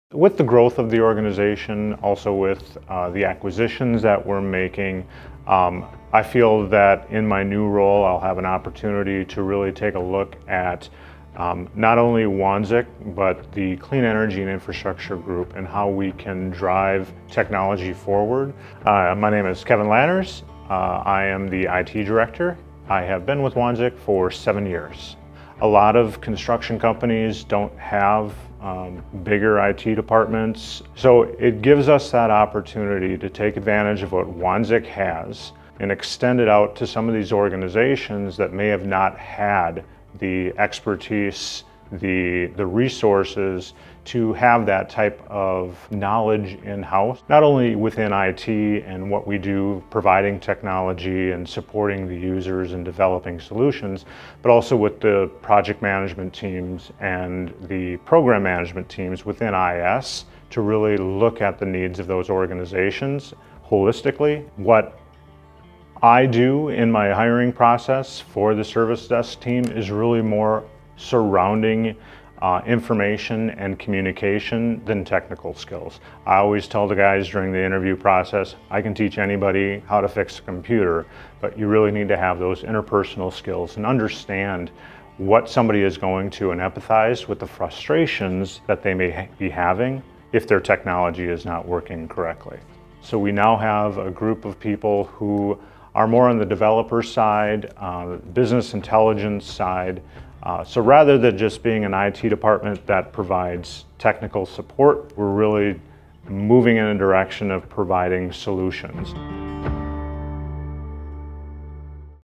Kevin Lanners becomes Director of IT
Kevin Lanners talks through his recent promotion and how Wanzek IT provides comprehensive solutions for Wanzek and